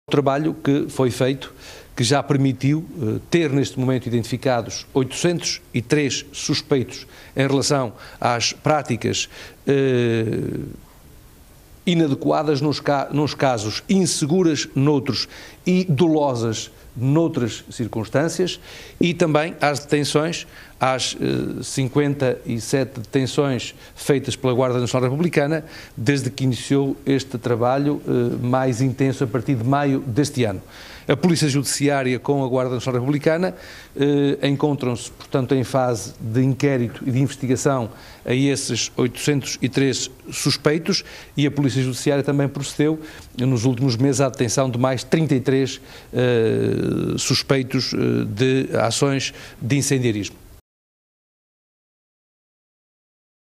Identificados 803 suspeitos de incêndios e 90 detidos pela GNR e PJ
Este ano foram identificados 803 suspeitos relacionados com casos de incêndios, que estão a ser alvo de inquérito e investigação, tendo a GNR e a PJ feito 90 detenções, revelou hoje o ministro da Administração Interna.

Edição de Luís Pedro Martins